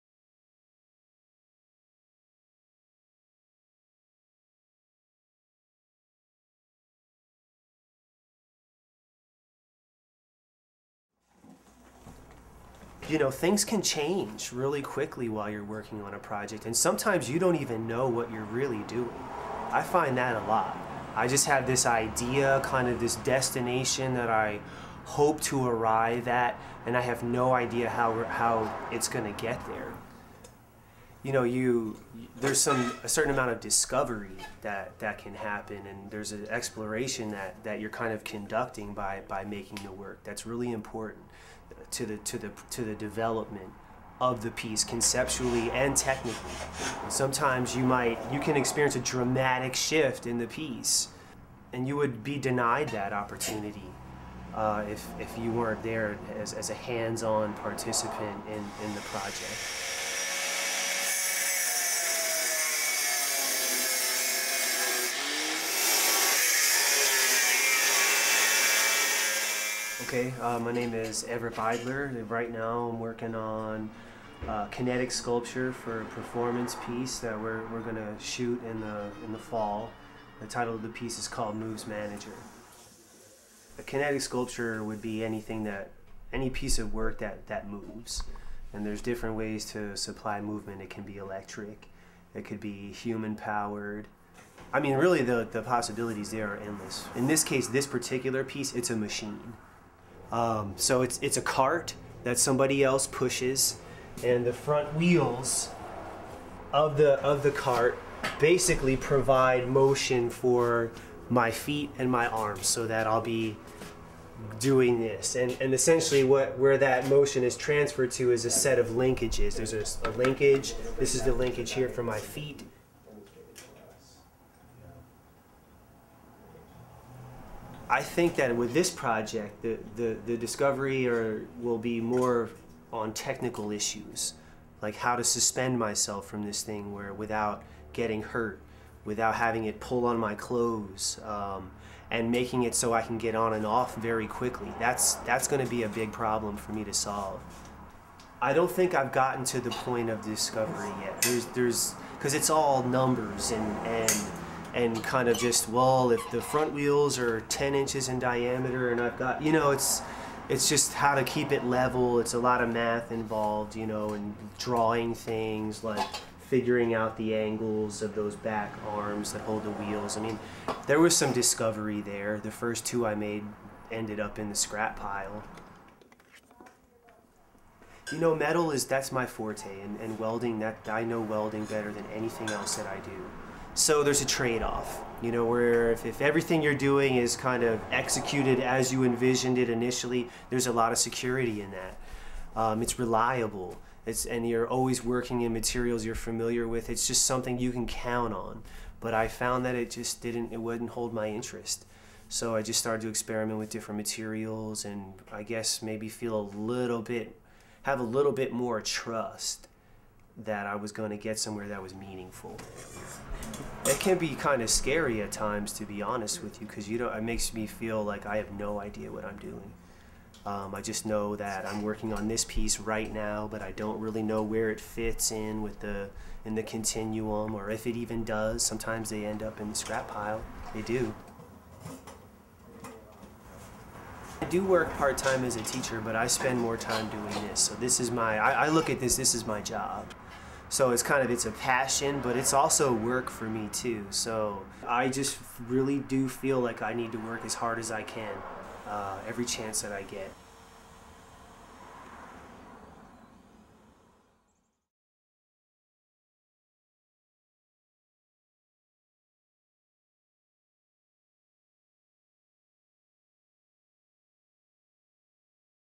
ART:PDX 2010 : Evertt Biedler : Sculptor & Media Artist
ART:PDX is a collaborative project of the NW Film Center's Young Filmmakers Program and  the Metropolitan Learning Center. Funded in part by the Regional Arts & Culture Council Project Grant (2010). MLC students interviewed 10 local Portland artists in this original series of video shorts.